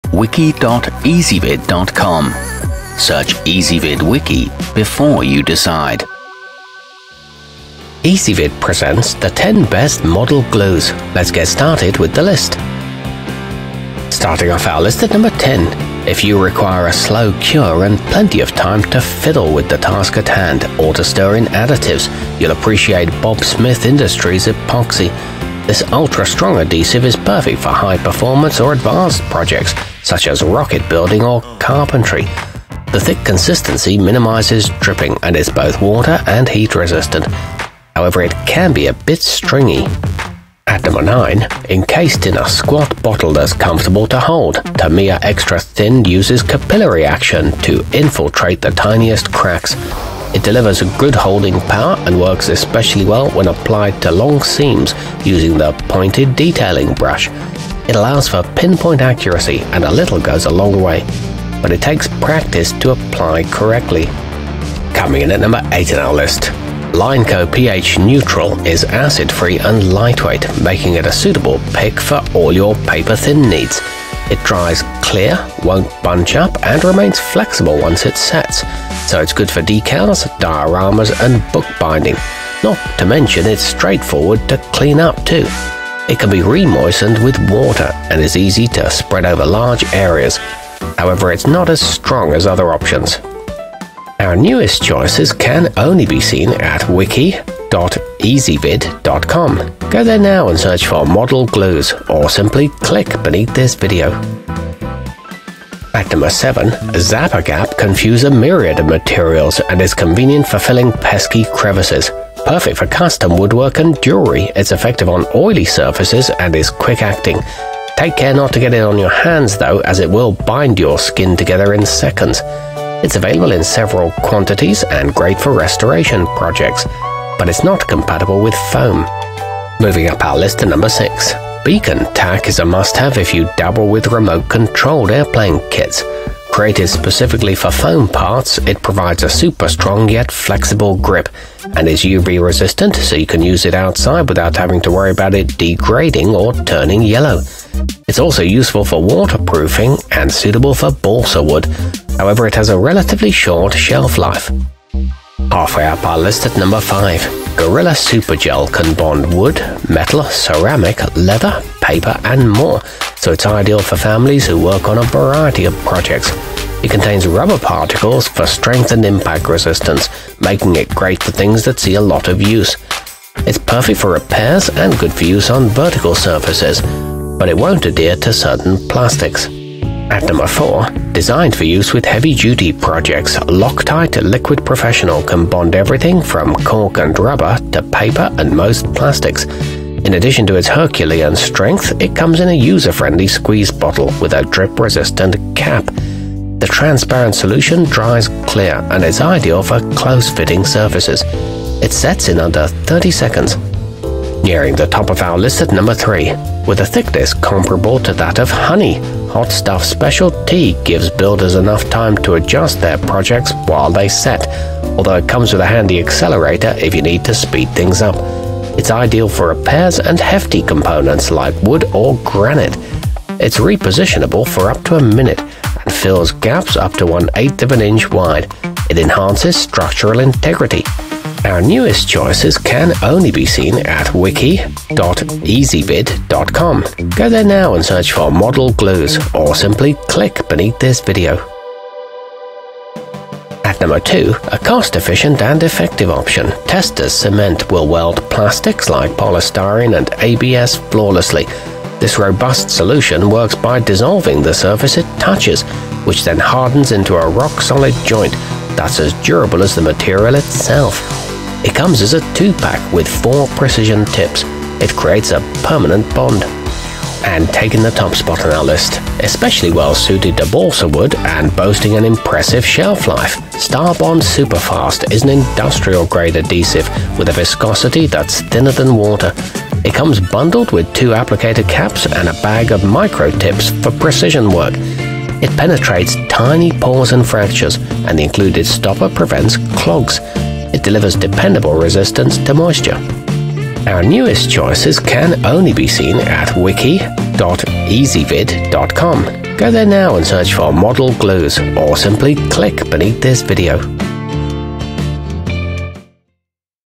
10 Best Model Glues 2020
Our complete review, including our selection for the year's best model glue, is exclusively available on Ezvid Wiki.

Model glues included in this wiki include the zap-a-gap, hot stuff special t, lineco ph neutral, beacon tac, starbond super fast, gorilla super gel, testors cement, loctite liquid professional, bob smith industries epoxy, and tamiya extra thin.